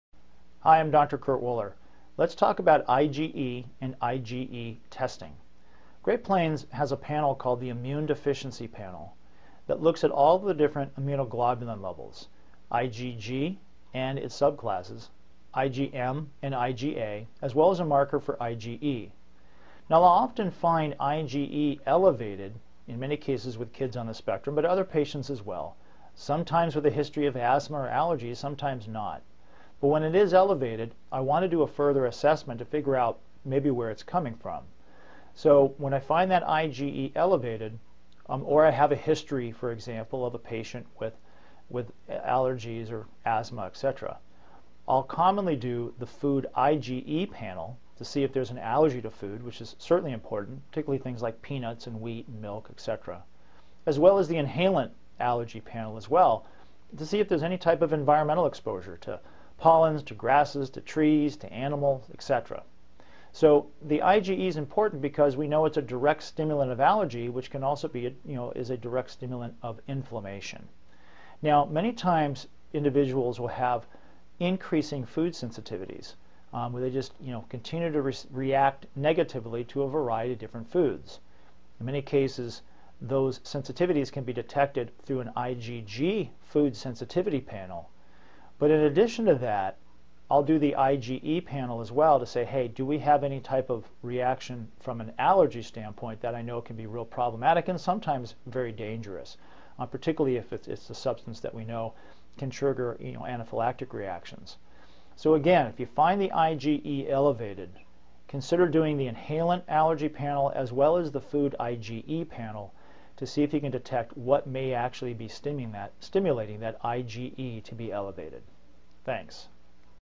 IgE Food and Inhalant Allergy Testing - Dr. Woeller
How, why, and when to use food allergy testing